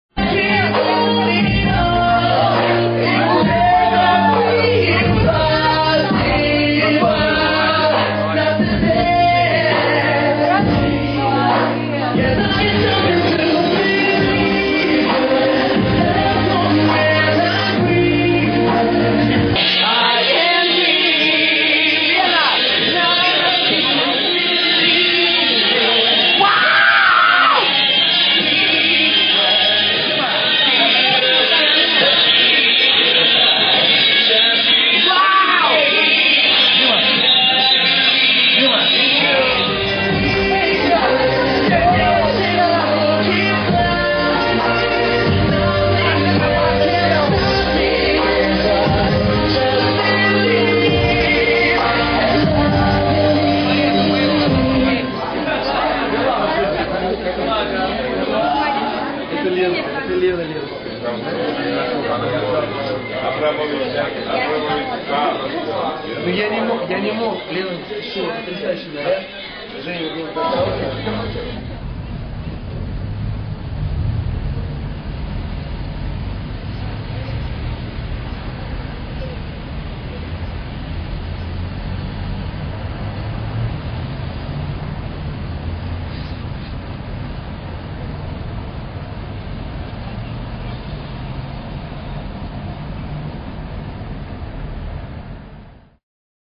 Дима Билан -- WITH LENA AFTER THE VICTORY IN EUROVISION
HOT KISS IN THE PARTY! THEY ARE SO HAPPY!